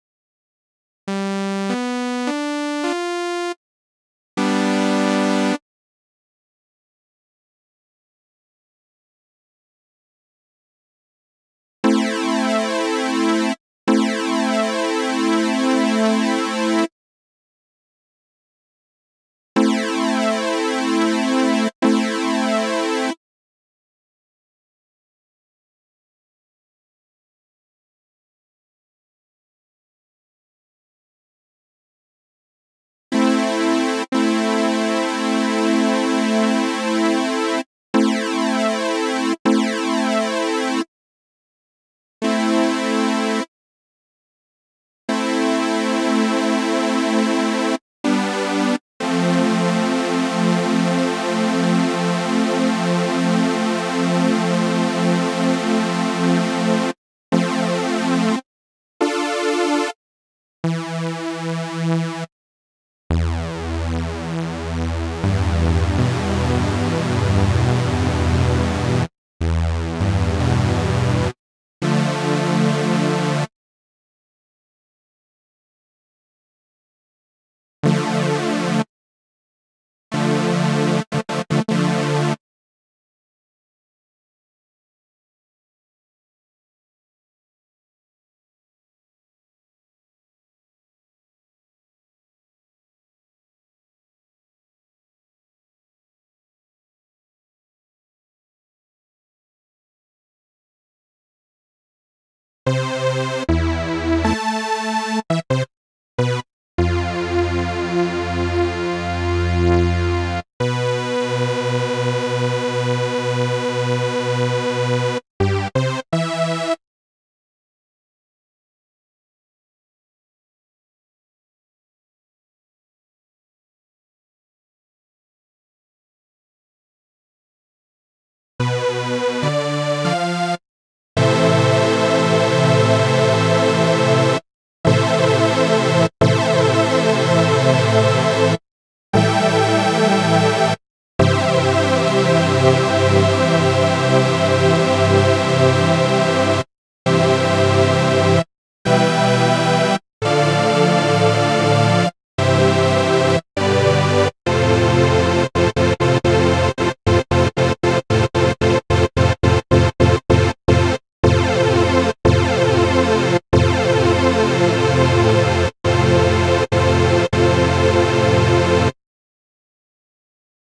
Nanostudio 2 Obsidian - how to add phase randomness to unison
Example how to add phase randomness to unison to make it sound less like  chorus, for standard oscillator and for sample oscillator.